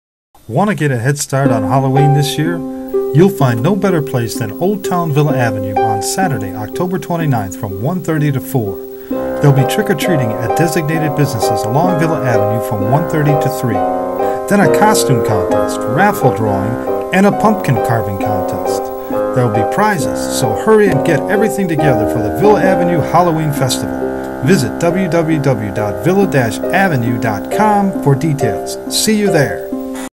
Old Town Villa Avenue Halloween Festival
The business owners along South Villa Avenue in Villa Park, Illinois are holding a Halloween Festival on Saturday - October 29th from 1:30 - 4:00 PM.
There will be trick-or-treating from 1:30 - 3:00 at designated businesses then a costume contest - raffle drawing - and a pumpkin carving and decoration contest at Cortesi Square.